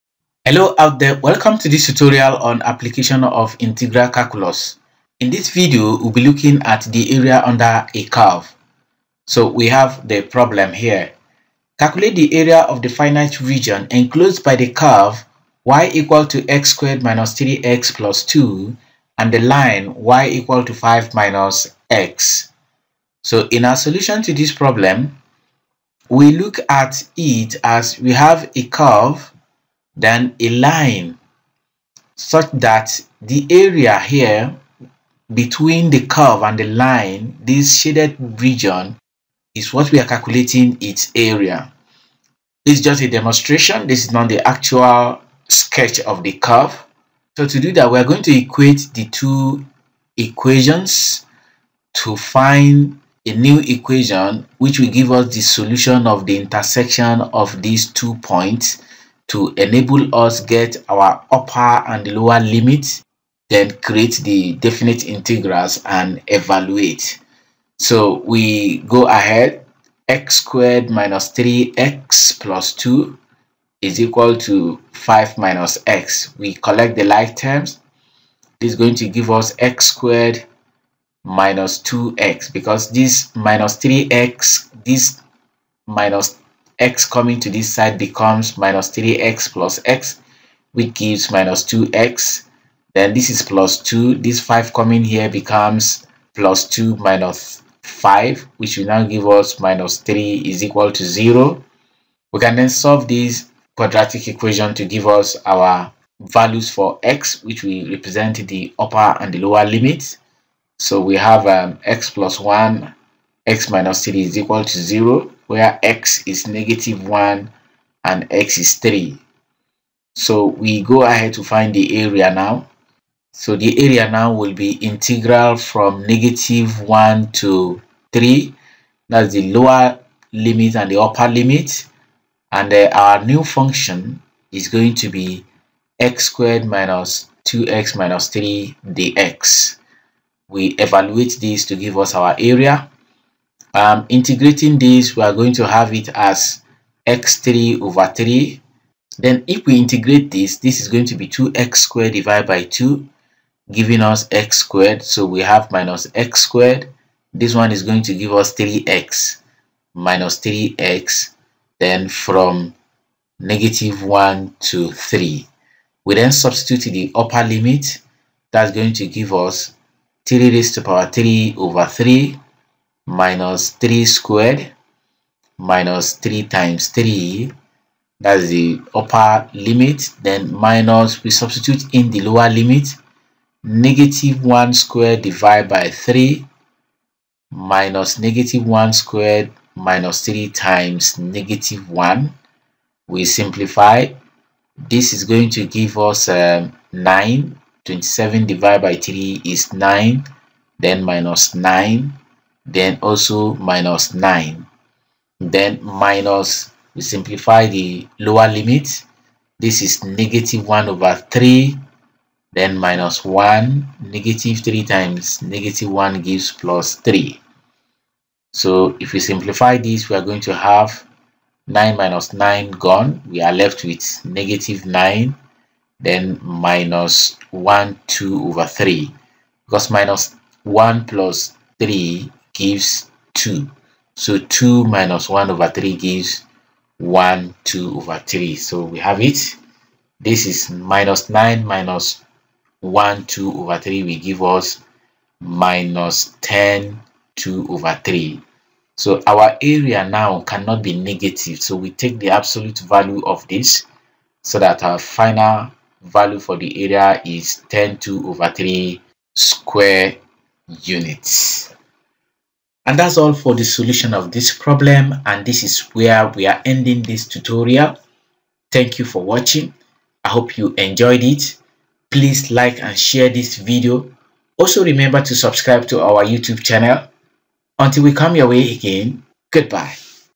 Application of Integral Calculus: The Area Under a Curve
The video covers how to calculate the area within a finite region of a curve and a straight line.